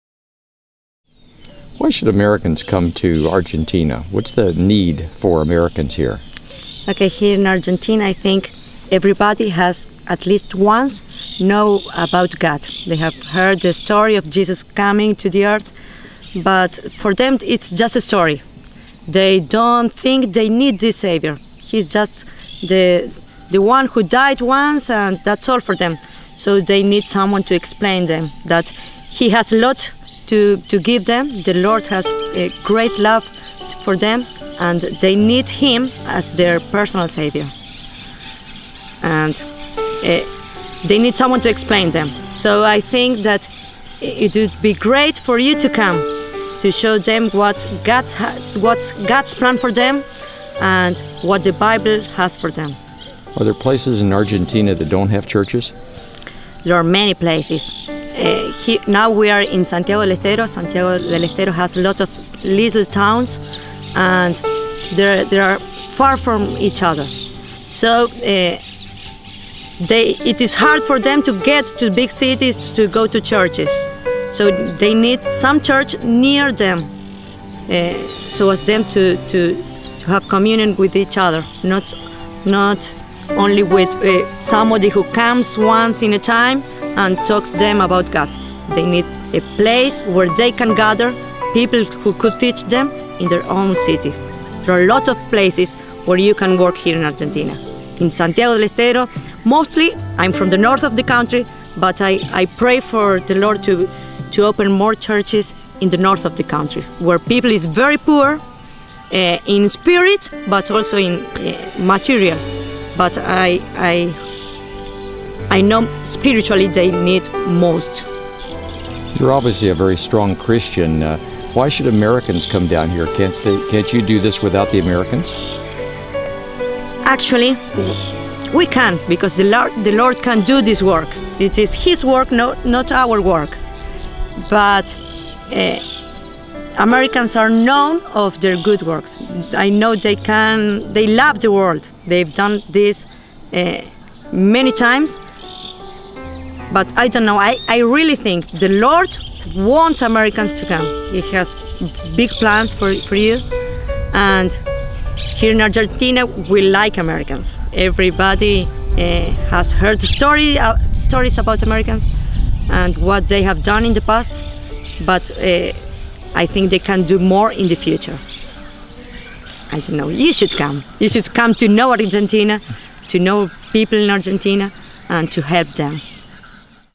Wanda (Argentina Translator) Testimony
She shares about North Americans coming down and working with the churches in Argentina to start churches in villages where there is no church.